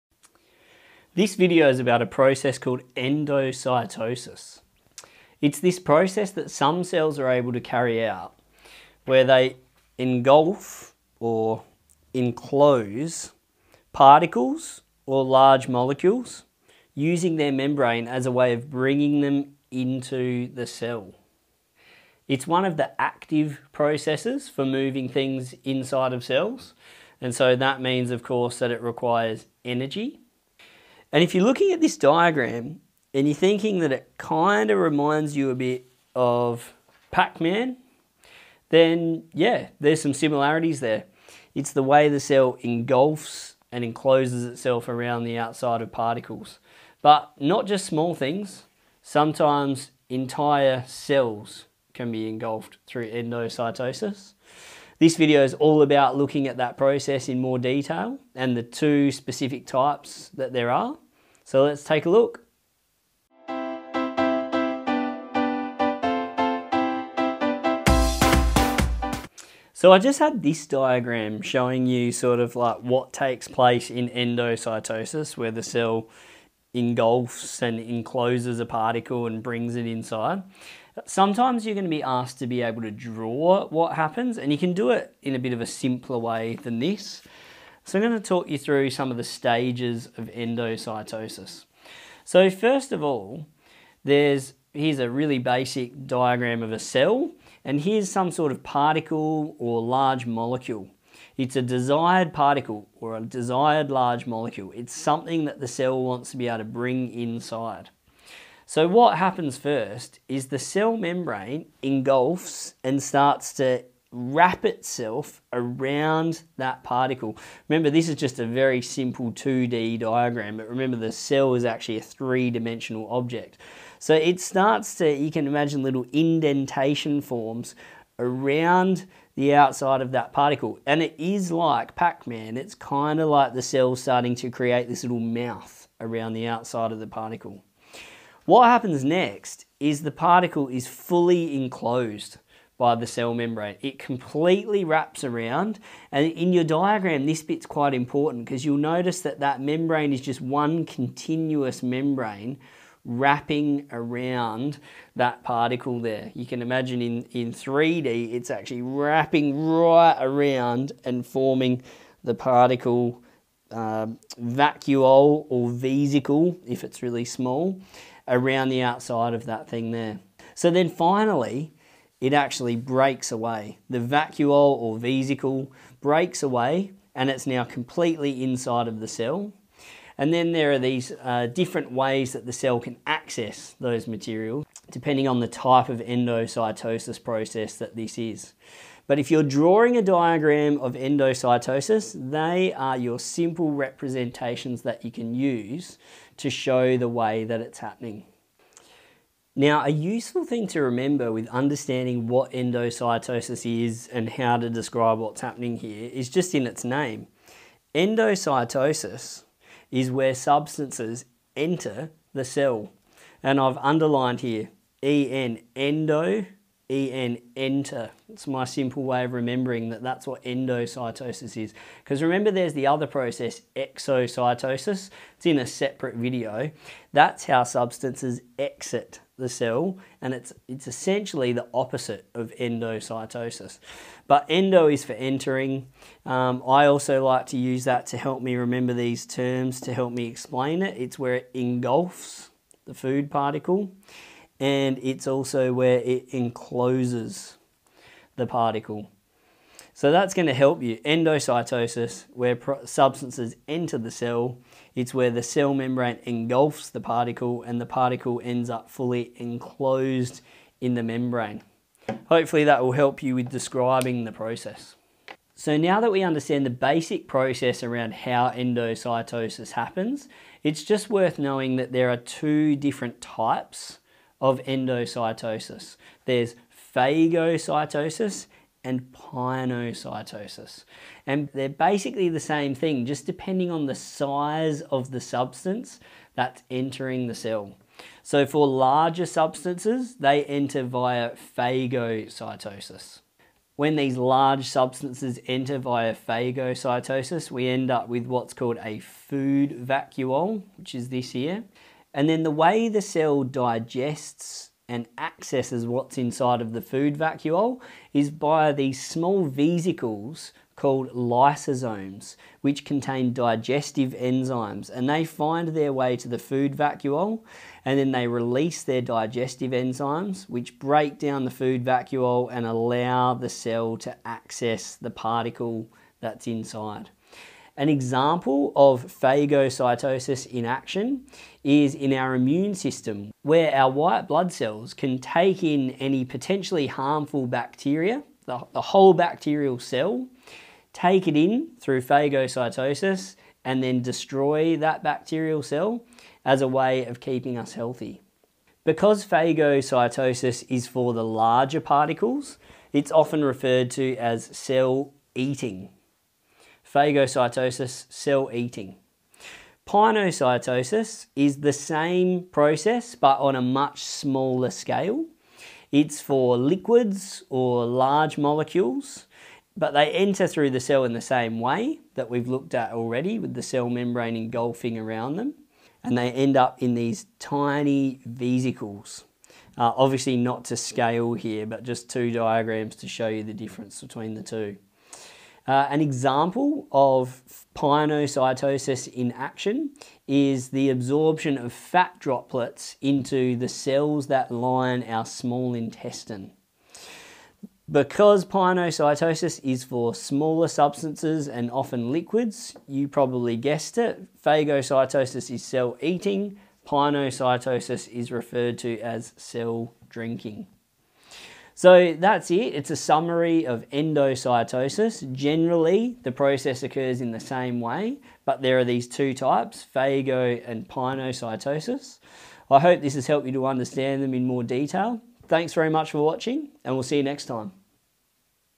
What is Endocytosis?
Endocytosis is an active process by which substances are taken into a cell by 'infolding' of the membrane. There are two types of endocytosis and they are called phagocytosis and pinocytosis.